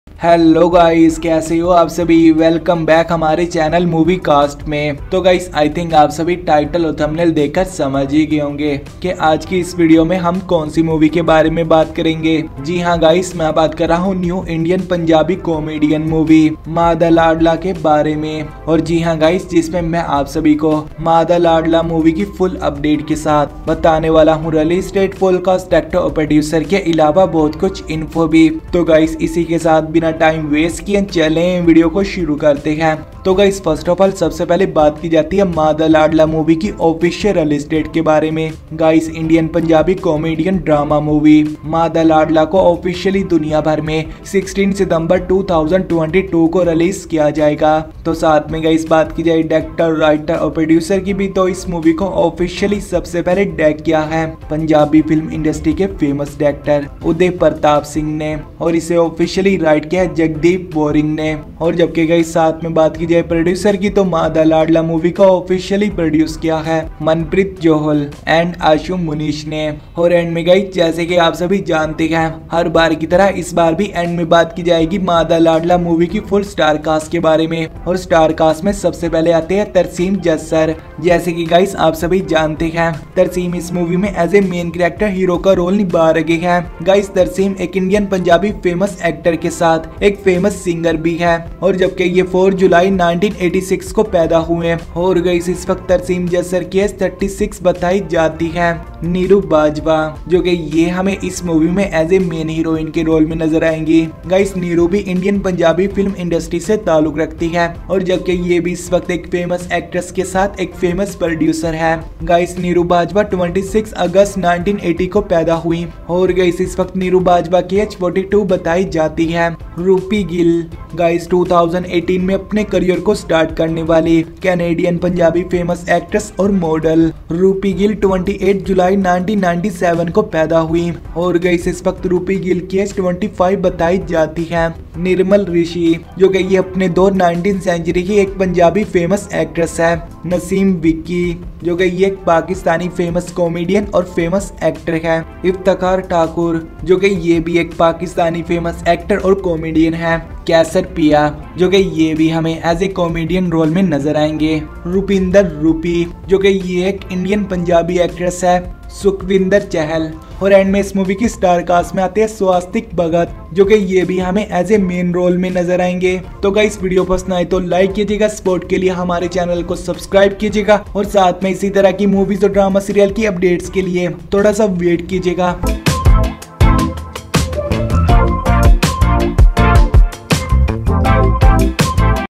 Maa Da Ladla Punjabi Movie Cast Full Name & More Review!!!
Maa Da Ladla Punjabi Movie Cast Full Name & More Review!!! | Movie Cast Channel

Maa Da Ladla (film) 2022 -  CHAPTER WISE
0:00 Movie Intro (Maa Da Ladla)
0:33 Maa Da Ladla Release Date?
0:48 Full Crew? (Director, Writer & Producer)
1:10 Maa Da Ladla Cast
2:46 Outro (More Latest Movies)

Maa Da Ladla 2022 & this Video full Info about...

* STAR CAST *
Tarsem Jassar
Neeru Bajwa
Roopi Gill
Nirmal Rishi
Naseem Vicky
Iftikhar Thakur
Qaiser Piya
Rupinder Rupi
Sukhwinder Chahal
Swastik Bhagat

* DIRECTOR *
Uday Pratap Singh

* WRITER *
Jagdeep Warring

* PRODUCER *
Manpreet Johal & Ashu Munish